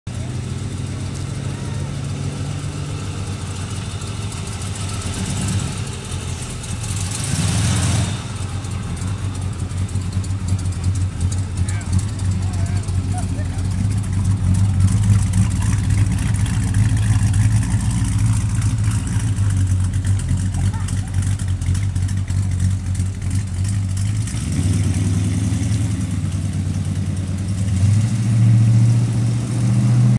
1968 Dodge Charger R/T
Leaving the 2015 Dayton Concourse d'Elegance after the show. This vehicle is a 440 automatic, last restored in 2000.
The paint is Ford color, maybe Purple Velvet.